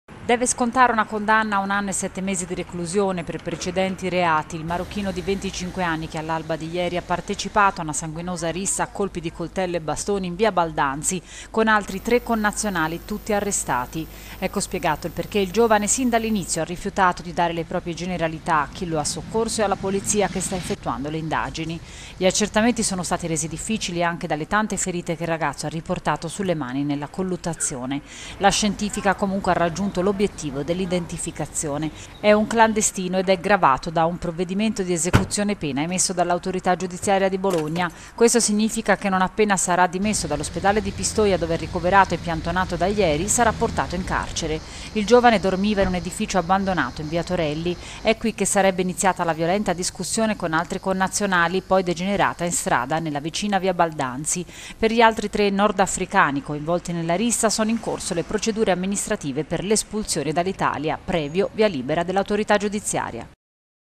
2018-07-21 PRATO - RISSA IN VIA BALDANZI, ESPULSIONE PER 3 DEI COINVOLTI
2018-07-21 PRATO - RISSA IN VIA BALDANZI, ESPULSIONE PER 3 DEI COINVOLTI